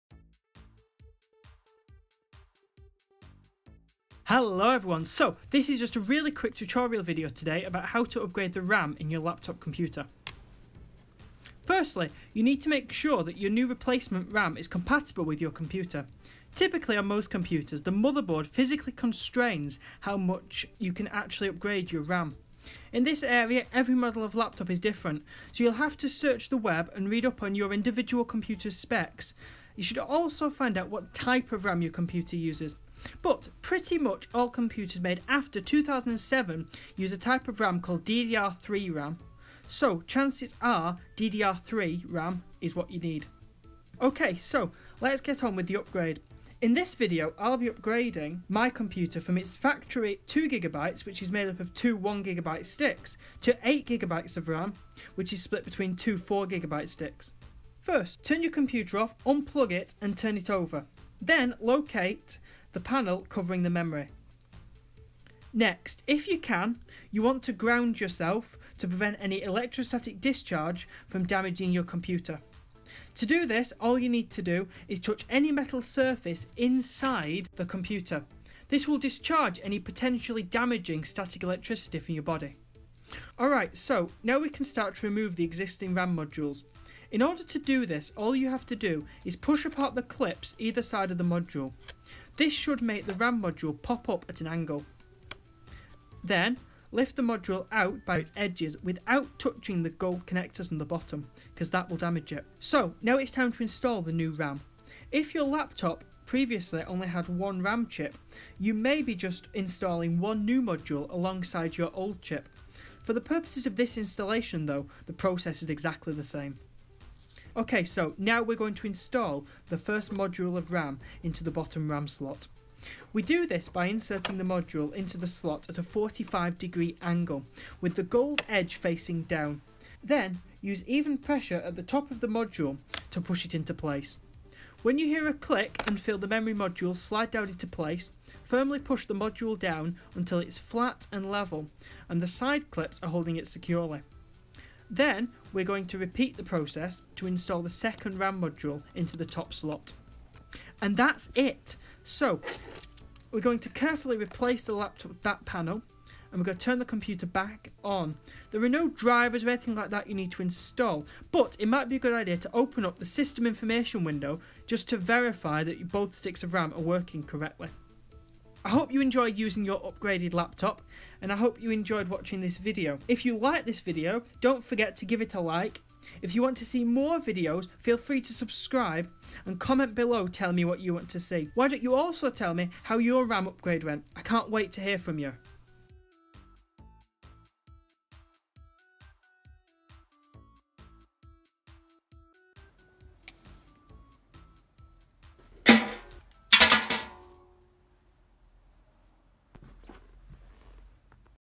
How to Upgrade the RAM (Memory) on a Laptop ~ Simple and Easy Tutorial
Got a sluggish or aging laptop that could use a speed boost? One of the most cost-effective upgrades to make your computer run faster is to simply add more RAM. In about the time it takes to read this step-by-step tutorial, you can upgrade your computer's memory and performance.